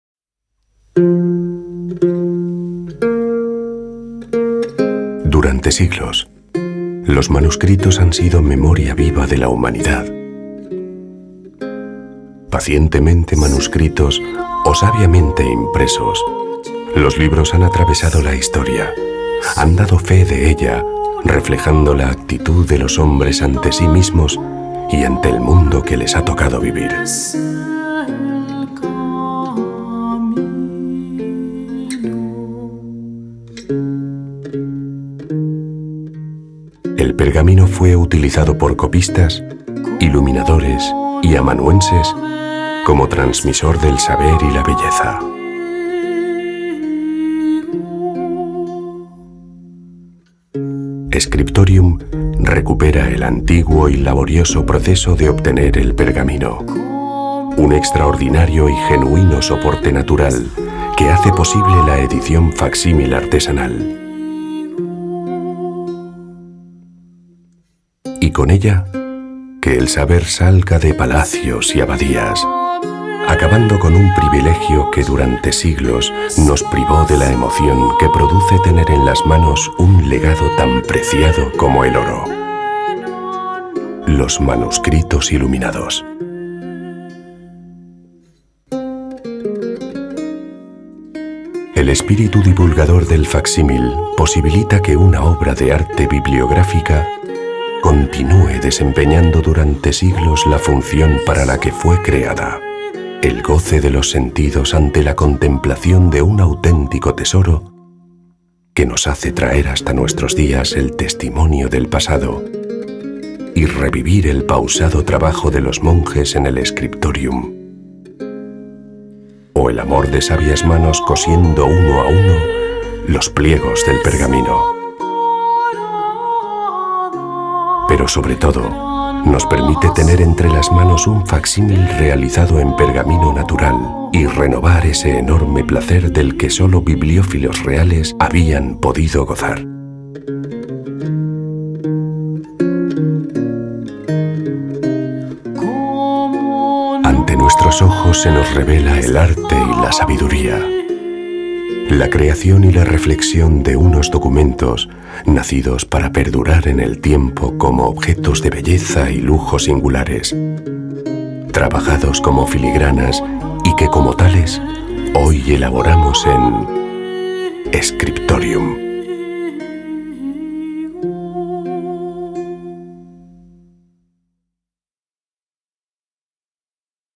SCRIPTORIUM
Scriptorium es una editorial que elabora facsímiles de gran calidad de manera artesanal, recuperando la antigua labor que durante siglos realizaron iluminadores, copistas y amanuenses.